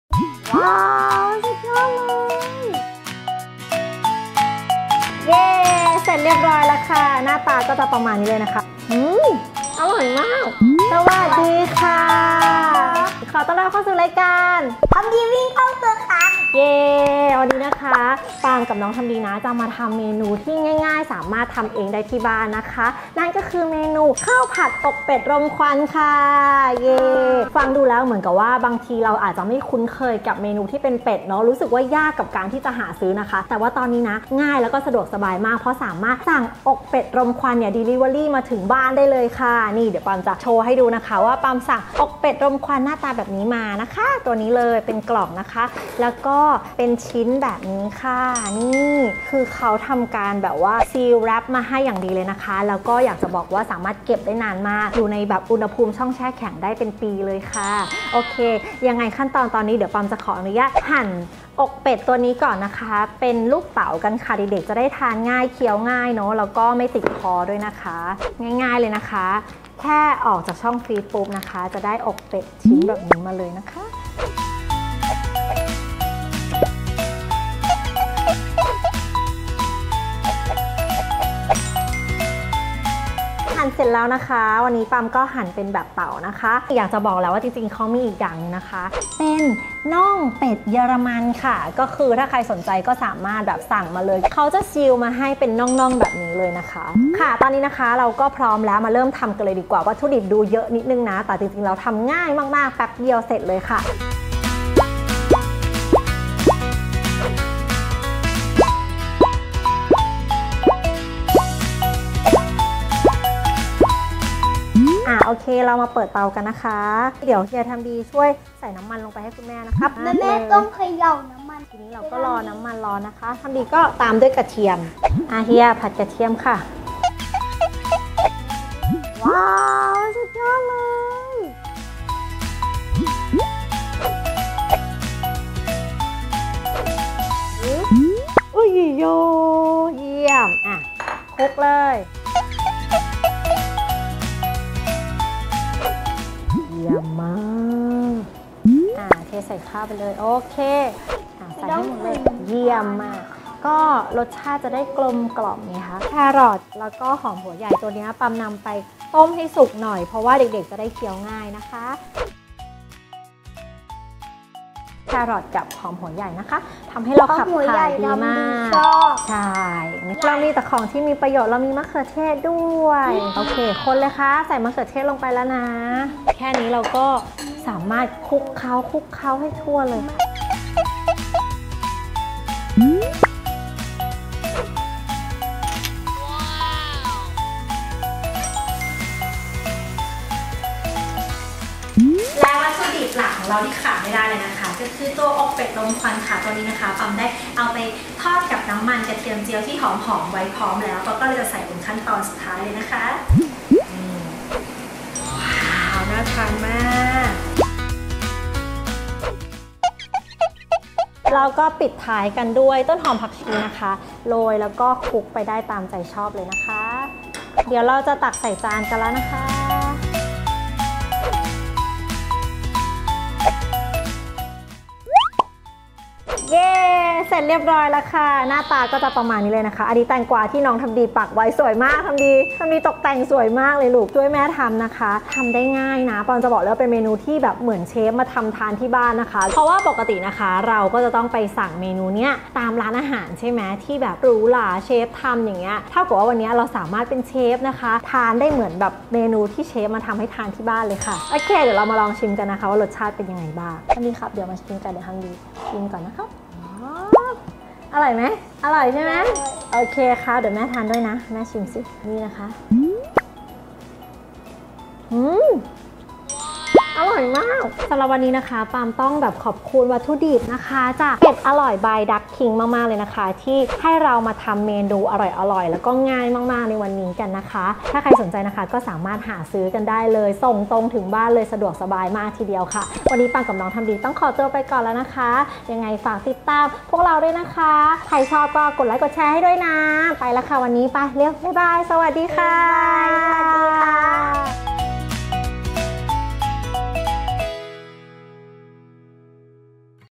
ธัมดีวิ่งเข้าครัว EP.8 : ข้าวผัดอกเป็ดรมควัน by DuckKing | คุณแม่เลอค่า
วันนี้ปาล์มชวนเฮียธัมดีมาเข้าครัวทำเมนูจากอกเป็ดรมควัน ไม่ต้องไปร้านหรูก็สามารถทำให้ลูกทานเองที่บ้านได้ง่าย ๆ ด้วย “อกเป็ดรมควัน by DuckKing” สามารถส่งวัตถุดิบคุณภาพดีให้ถึงครัวที่บ้าน เอาไปทำเมนูอะไรก็อร่อย รสชาติเลิศ ถูกปากลูก เชฟธัมดีการันตีค่ะ

บ้านไหนที่อยากสั่งมาลองทาน ปาล์มแจกส่วนลด 10% เพียงพิมพ์โค้ด MLDK ที่ สอบถามเพิ่มเติม LINE OA